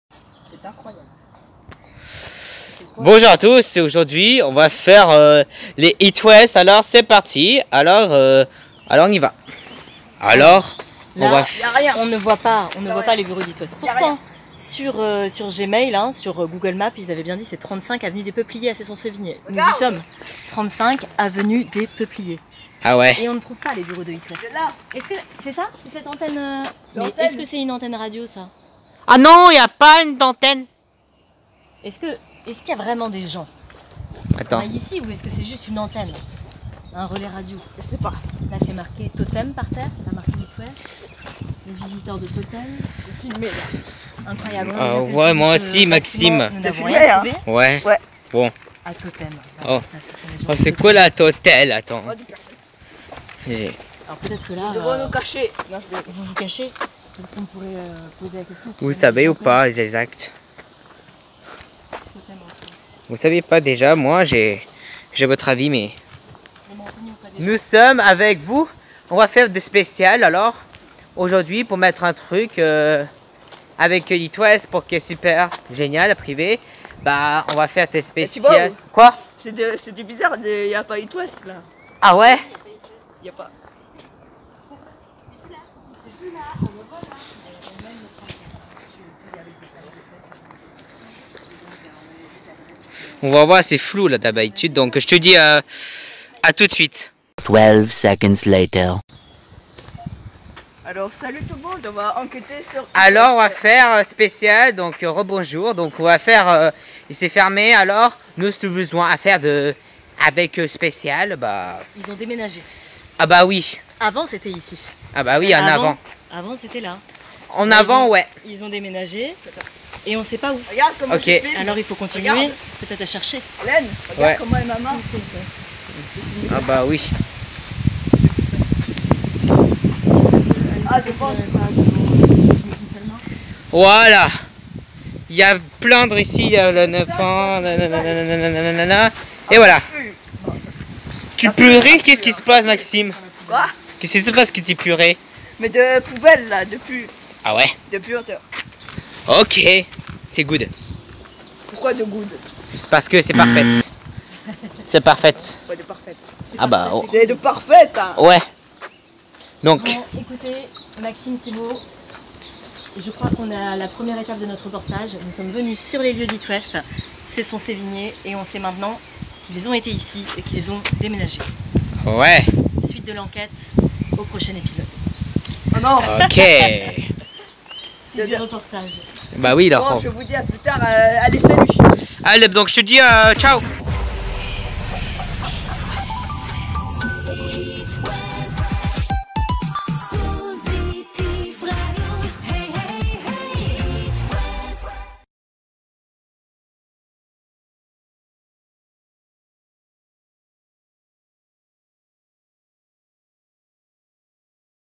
ON A PERDU DE HIT WEST ET DÉMÉNAGER !!!
On à fermé de stations la radio de Hit West donc Maison et déménager de la sortie je souhaite pour votre radio de Hit West Positive Radio !